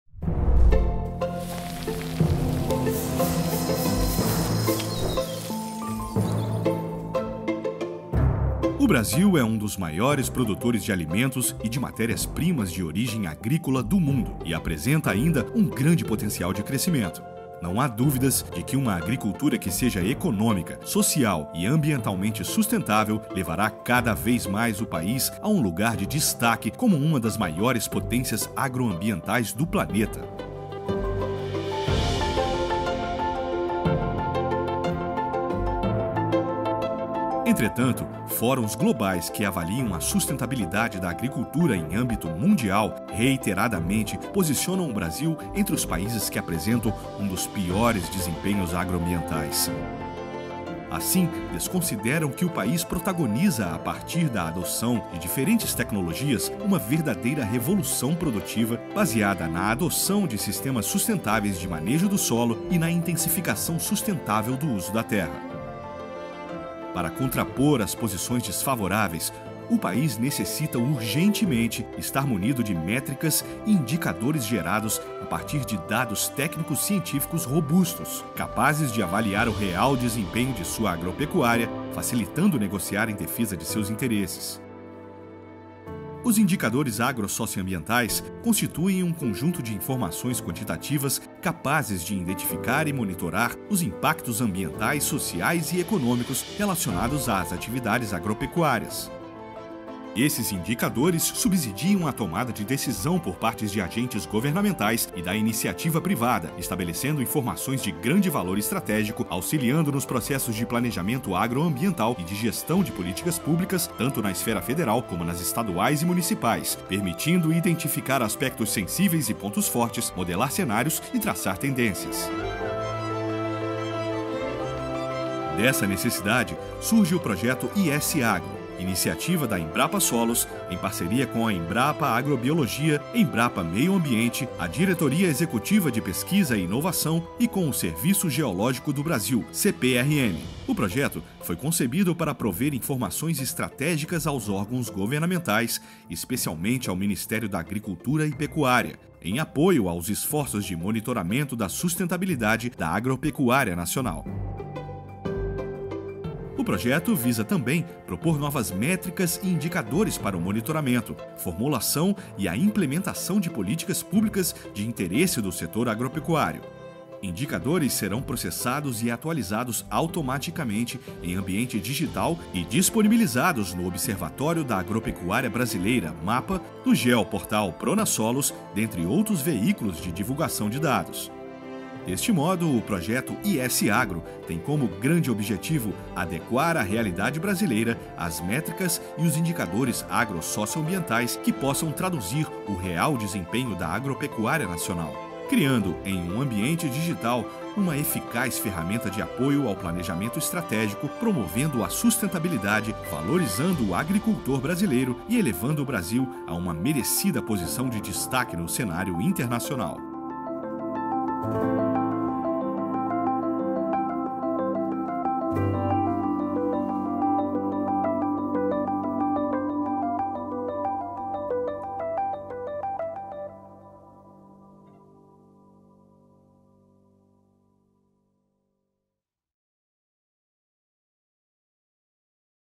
Projeto IS_Agro
O Projeto IS-Agro é uma iniciativa da
 Embrapa Solos em parceria com a Embrapa Agrobiologia,
 Embrapa Meio Ambiente, Embrapa DEPI/SPNG e o Serviço
Geológico do Brasil (SGB/CPRM). O projeto foi concebido para
prover informações estratégicas aos órgãos governamentais,
 especialmente ao Ministério da Agricultura e Pecuária
(MAPA), em apoio às iniciativas de monitoramento da
sustentabilidade da agropecuária nacional.

O projeto tem por objetivo avaliar e adaptar metodologias
propostas em fóruns globais e propor novas métricas e
indicadores que possam traduzir o real desempenho da
agropecuária nacional. Em um ambiente digital, indicadores
agro-socioambientais serão processados automaticamente,
continuamente atualizados e disponibilizados, na forma
de dashboards, em um serviço web consumido pelo
Observatório da Agropecuária Brasileira (MAPA), pelo
Geoportal PronaSolos ou por qualquer outro veículo
institucional de disponibilização de dados.